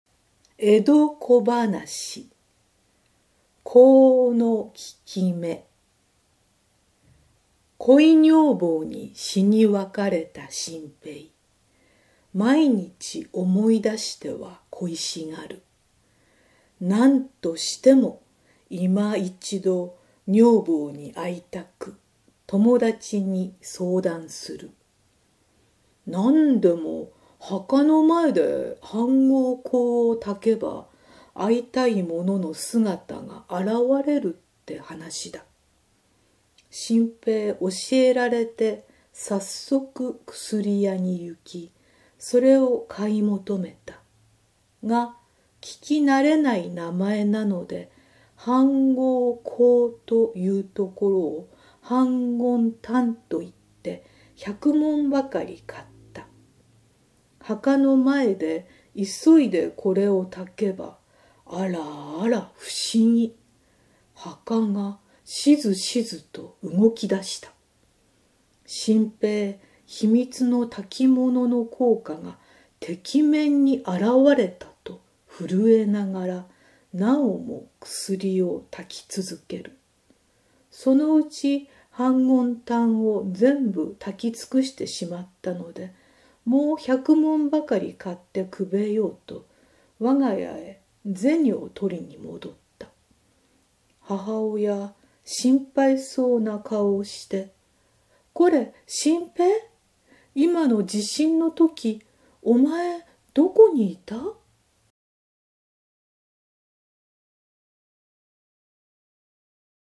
（江戸小噺）香の効きめ 朗読：向井智子
恋女房に死に別れた信平は　何としても今一度女房に会いたいと友だちに相談します。反魂香（はんごんこう）を焚けば、会いたい者の姿が現れると教えてもらった信平は早速薬屋に行きますが、聞き慣れない名前のため、間違って反魂丹（はんごんたん）を100文買います。墓の前で急いで焚いたら、墓が動き出したのです。もうすぐ恋女房に会えると喜んだ信平は買った反魂丹がなくなるまで焚き尽くしました。恋女房は現れないので、もう100文追加で買おうと自宅に財布を取りに帰ると、母親が心配顔で「今の地震の時、お前どこにいた？」。

反魂香というのは、焚くとその煙の中に死んだ者の姿が現れるという伝説上の香。もとは中国の故事にあるもので、中唐の詩人・白居易の『李夫人詩』によれば、前漢の武帝が李夫人を亡くした後に道士に霊薬を整えさせ、玉の釜で煎じて練り、金の炉で焚き上げたところ、煙の中に夫人の姿が見えたといわれています。一方､反魂丹は、食傷、腹痛などに効く丸薬。香ではありません。霊験あらたかな反魂香のおかげで、恋女房の墓が動いたと思い込んだ信平は、もっと焚けば、逢えると思ったのですが、地震が起きたせいだったのですね。
江戸時代の財布はどういうものかしらと調べてみました。画像を見て、私はなるほどと納得しました。財布の紐をしめるのは、こういうことだったのですね。時代劇を見ていると、スリのことを巾着切りと言いますから､巾着袋に小銭を入れることもあったのでしょうね。巾着もやはり、紐をしめますね。

ご興味のある方は　古今亭志ん朝師匠の落語『反魂香』をぜひご覧下さいませ。コロナ渦が落ち着いたら、浅草演芸場に行き､大好きな落語を堪能したいと思う今日この頃です。
・落語「反魂香」の舞台を歩く
・反魂丹は、江戸時代に製造が始まった、富山を代表する薬です。動・植物性の生薬を効果的に組み合わせて作られており、現代人のストレスからくる胃腸障害、食べ過ぎ飲み過ぎによる各症状、消化不良などにすぐれた効果を発揮するとのことです。
・江戸時代のお財布って、どんなやつ？